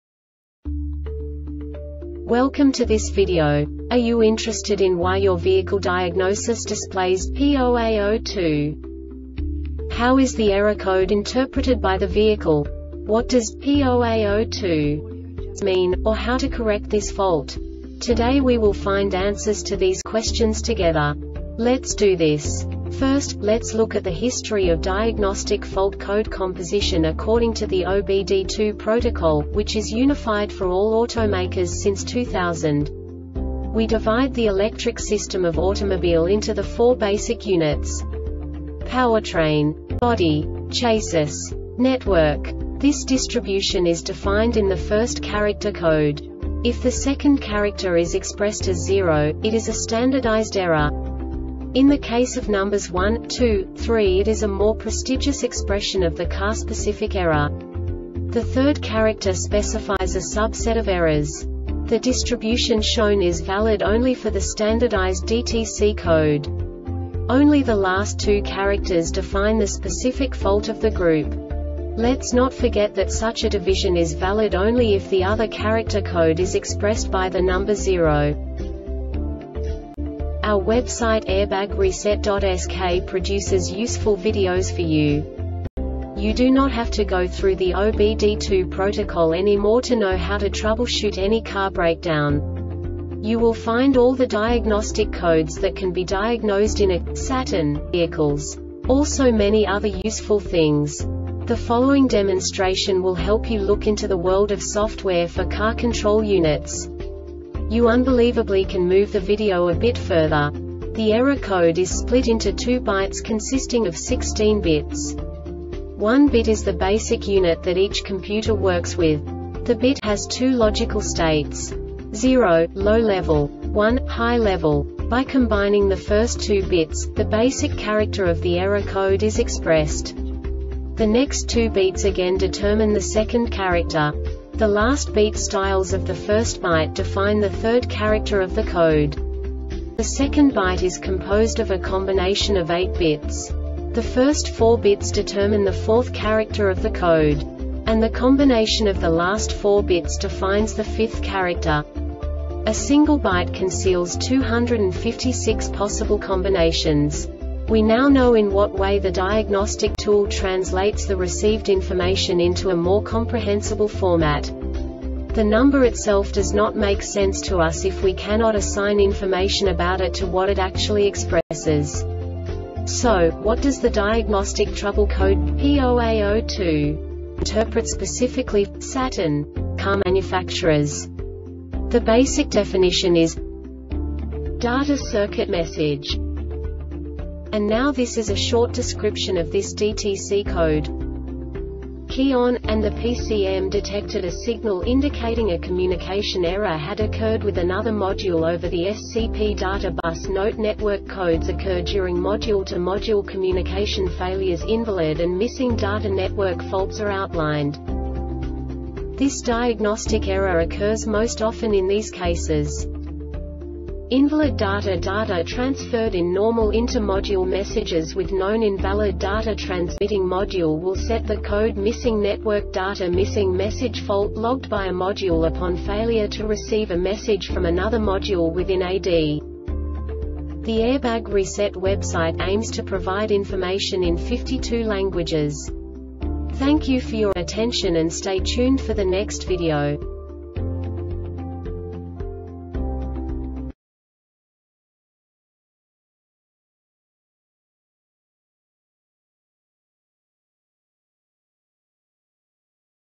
DTC Saturn P0A02 Short Explanation
Contents:
0:21 Basic DTC analysis according to OBD2 protocol standard.
1:48 Insight into programming
2:58 A diagnostically understandable description of the Diagnostic Trouble Code
3:05 Basic error definition

Make:
Saturn
Code:
P0A02
Definition:
Data Circuit Message
Description:
Key on,and the PCM detected a signal indicating a communication error had occurred with another module over the SCP data bus.Note: Network codes occur during module-to-module communication failures.Invalid and Missing data network faults are outlined b
Cause: